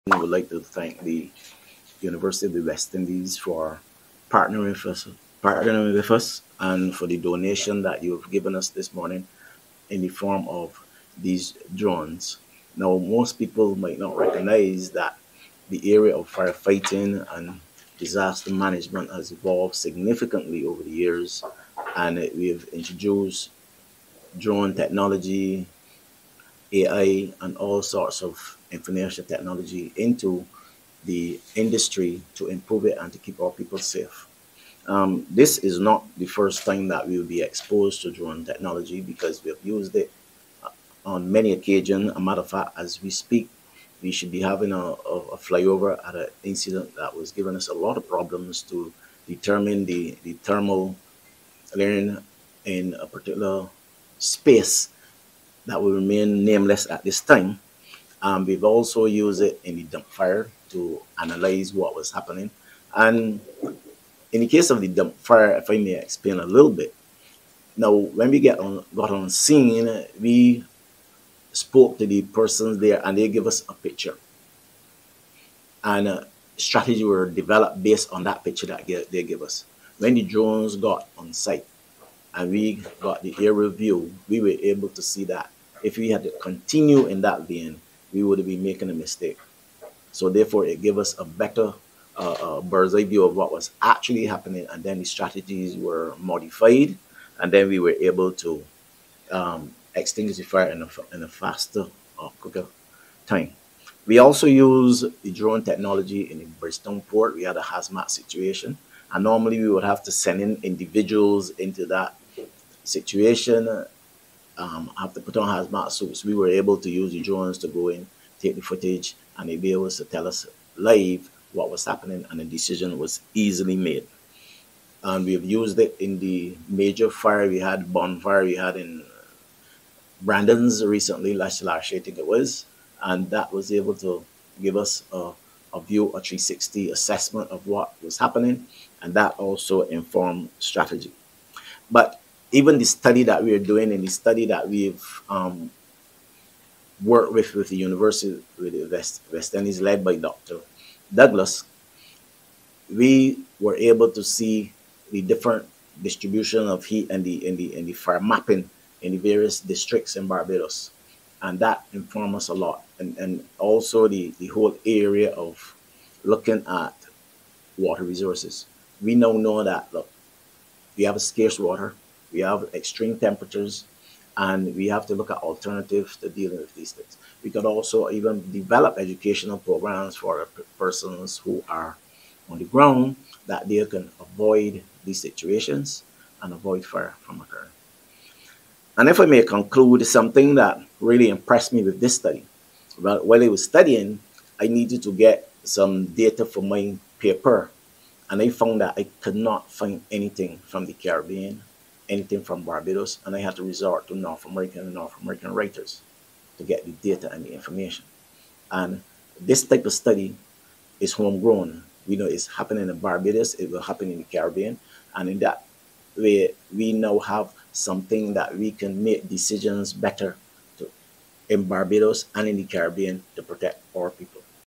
Fire support: Barbados Fire Service gets drones from UWI to help fight blazes
The Barbados Fire Service (BFS) is now better equipped to fight the most difficult blazes.

It not only received a donation of four drones on Wednesday but some fire officers are participating in an 18-month training exercise aimed at assessing the correlation between climate change and wildfires in Barbados.

During a presentation ceremony at the Centre for Biosecurity Studies at the University of the West Indies (UWI), Cave Hill Campus, Chief Fire Officer Errol Maynard said that advancements in technology have helped officers carry out their duties more effectively and precisely.

🎥: BT’s Remy Rock